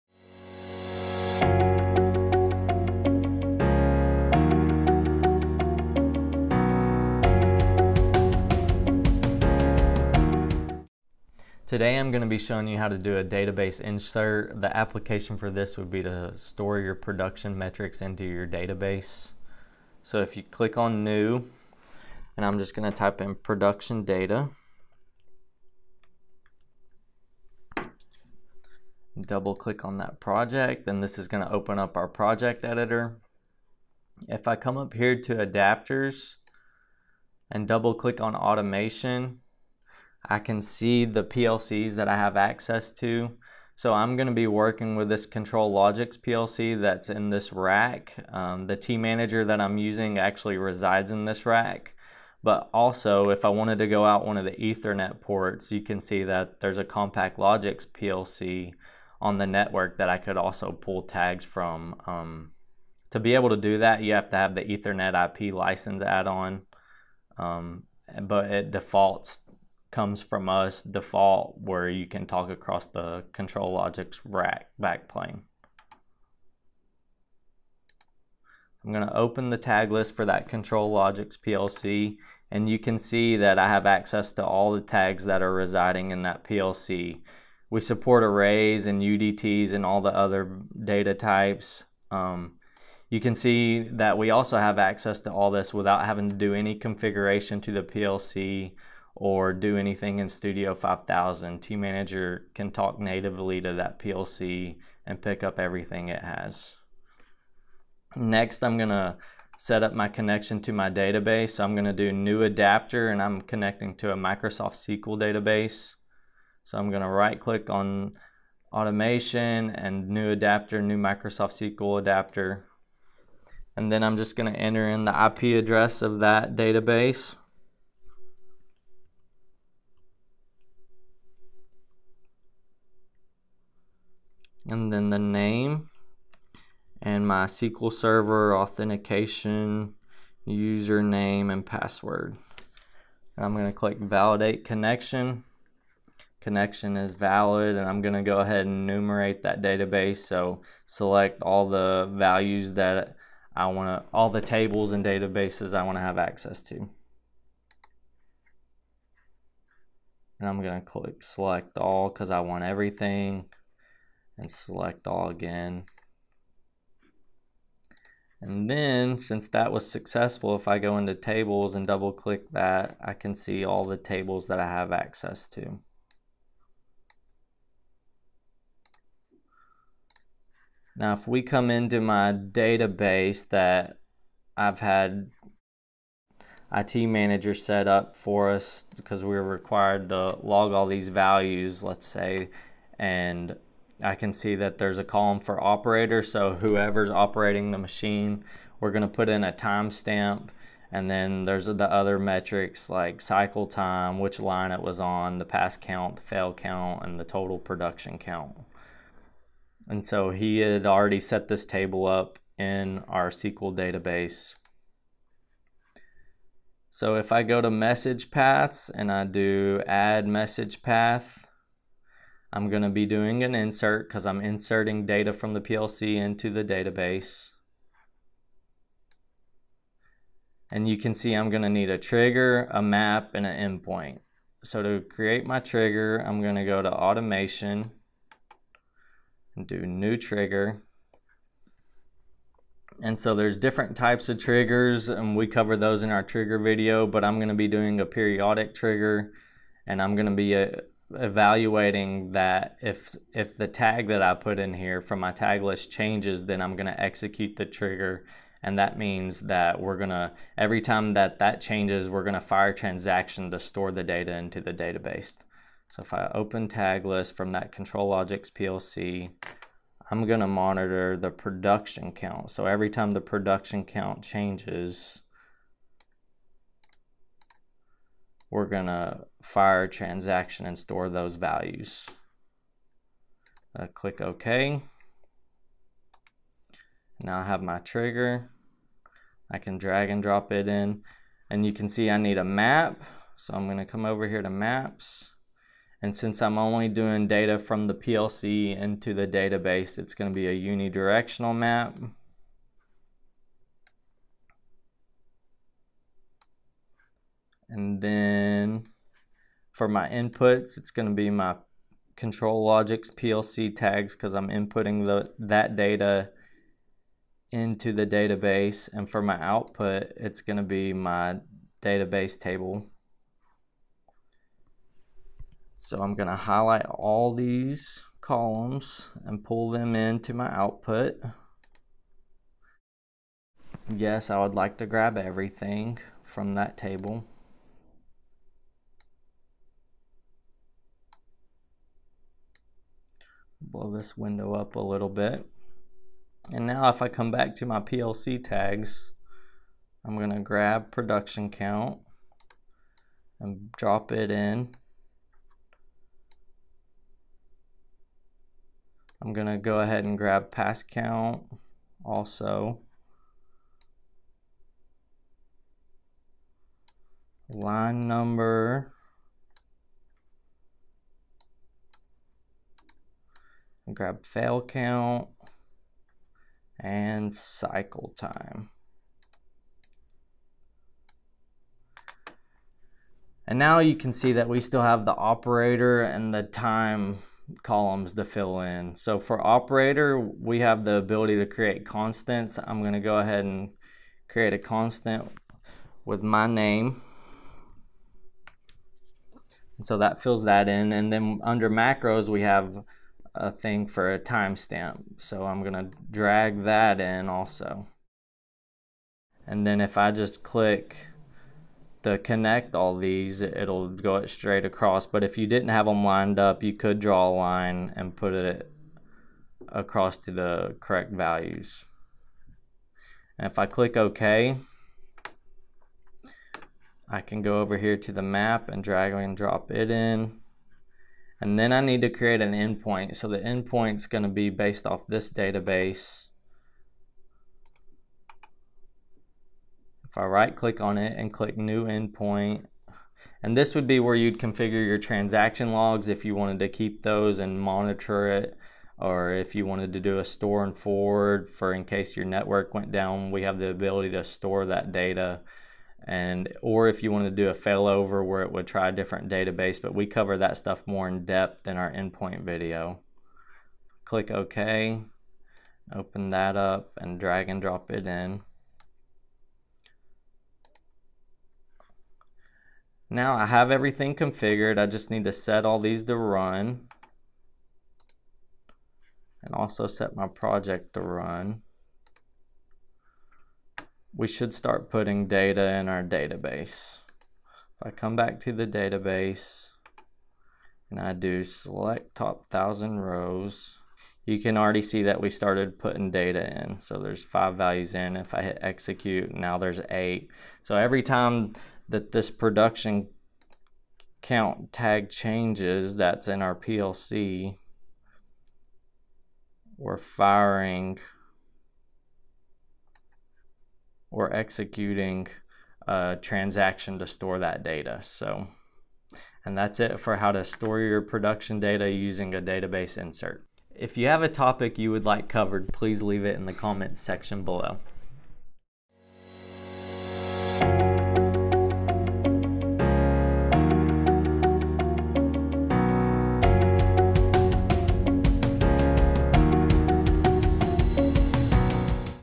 tManager -- How to Store PLC Tag Values in an SQL Database
Use tManager to monitor PLC tags and store values in an enterprise SQL database.  Reduce load and memory on your PLC and leverage the power of your enterprise SQL database with tManager handling the transactions.